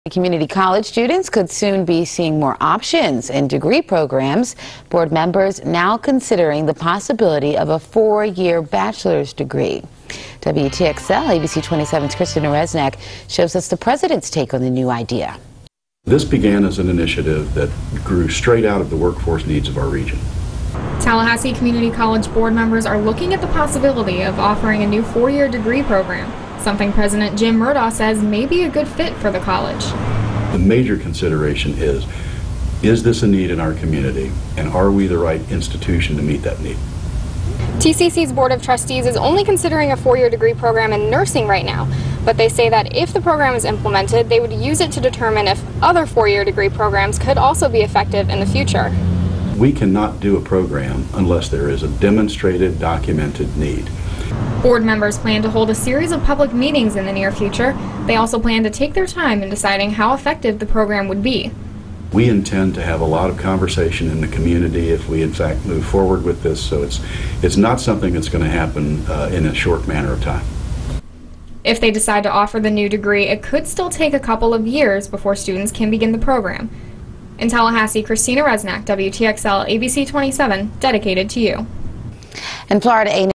TCC considers four year degree program
Board members of Tallahassee Community College are considering adding a four year degree program.